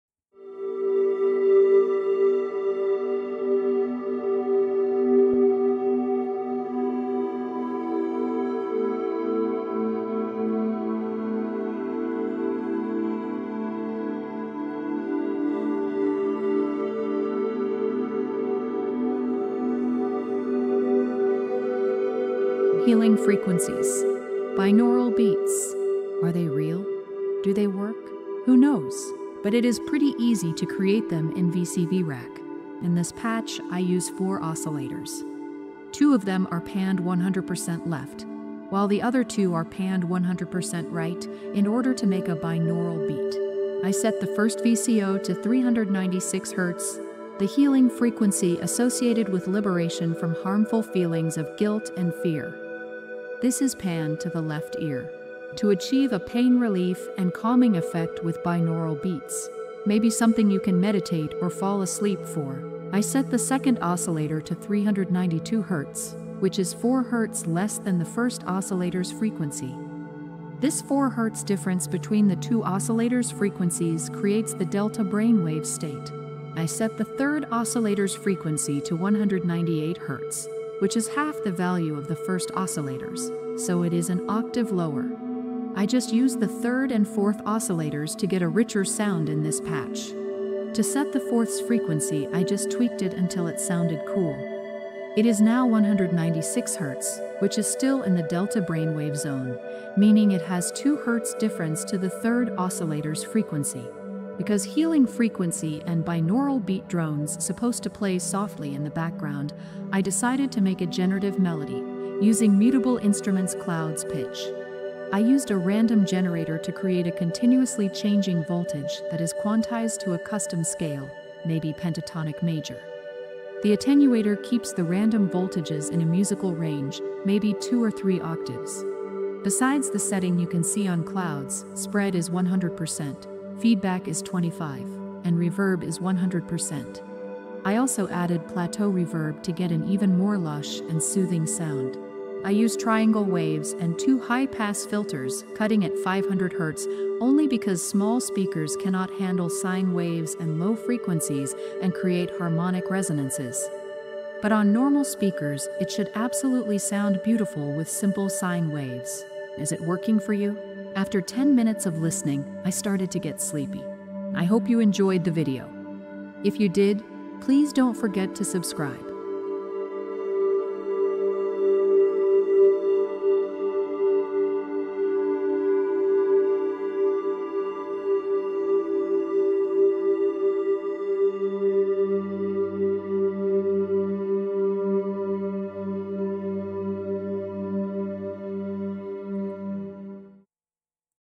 Creating a Healing Frequency and Binaural Beats Patch in VCV Rack | Tutorial
Welcome to my VCV Rack tutorial where we dive into the world of healing frequencies and binaural beats! In this guide, I'll explore how to harness the power of sound to induce relaxation, focus, and inner balance using VCV Rack, the open-source virtual modular synthesizer.

This tutorial is designed to provide step-by-step instructions to create your own custom healing frequency and binaural beats patches.

Let's dive in and start creating beautiful sounds together!